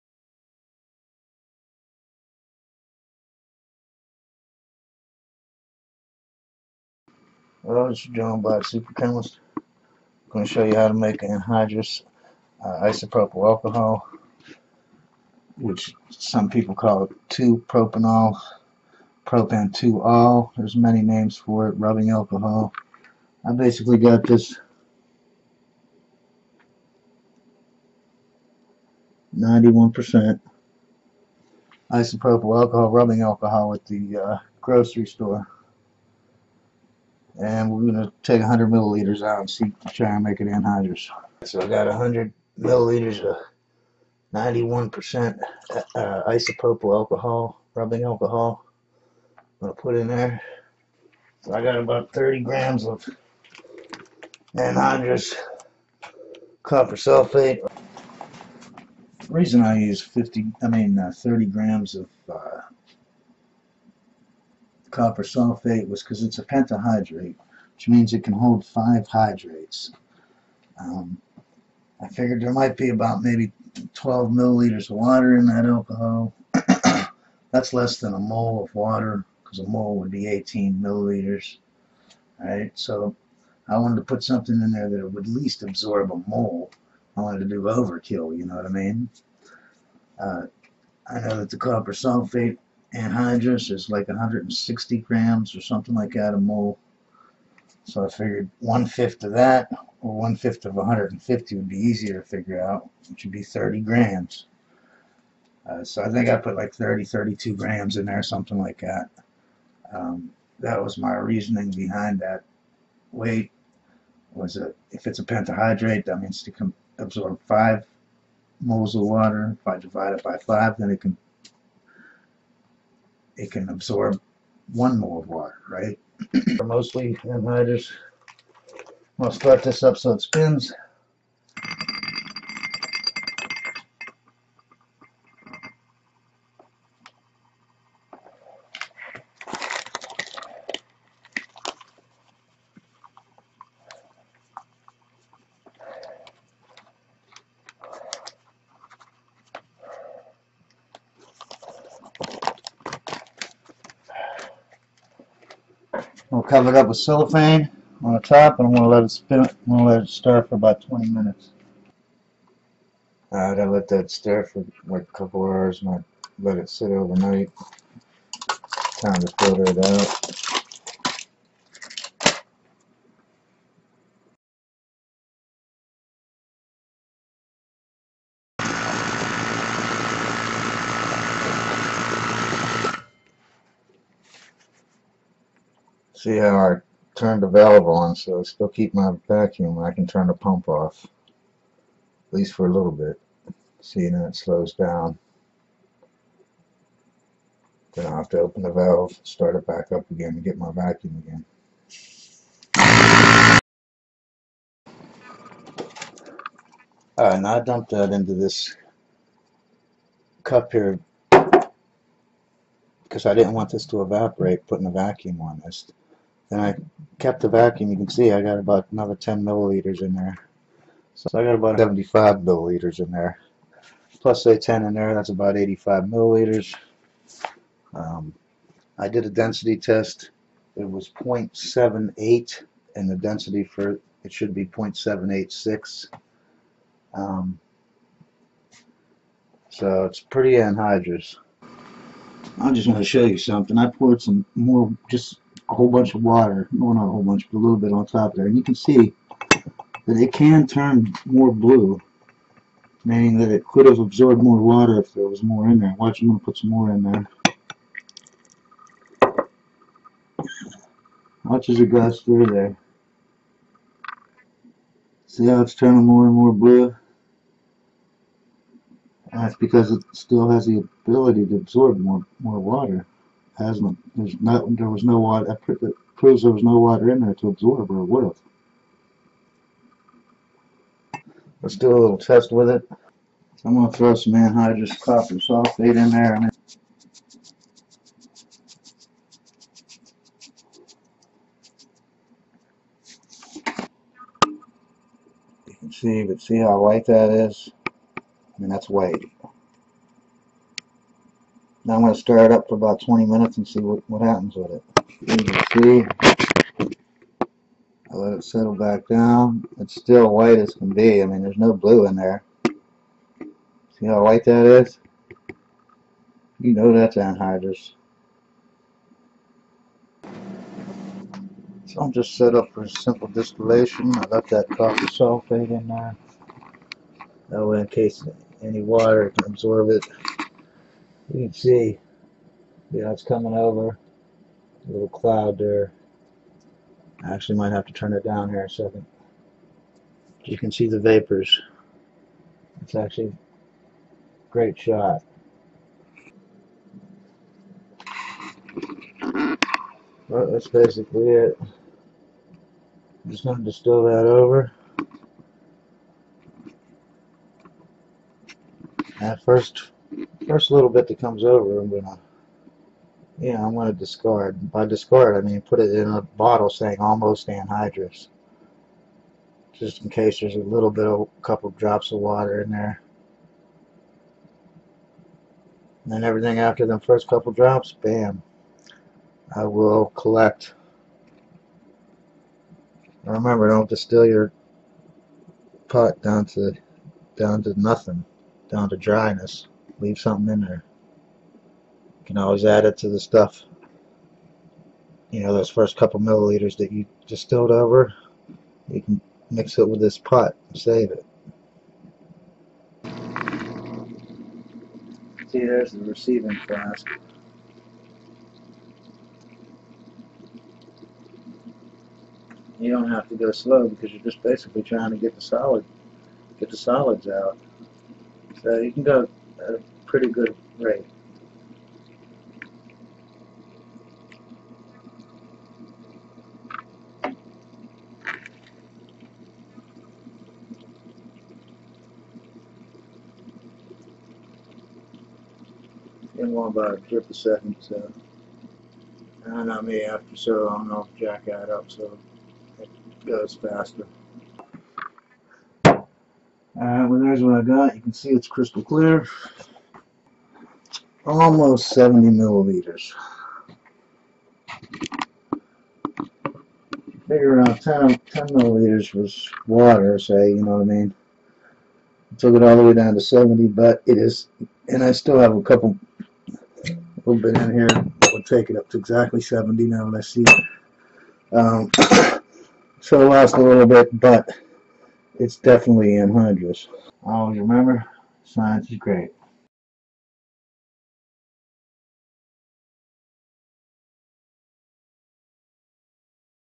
MAKE ANHYDROUS ISOPROPANOL
This video i actually do the work in the lab and i made rubbing alcohol into  PURE  ANHYDROUS ISOPROPANOL.   Isopropanol does not hold on to the water is much as ethanol so it is easier to dry and make anhydrous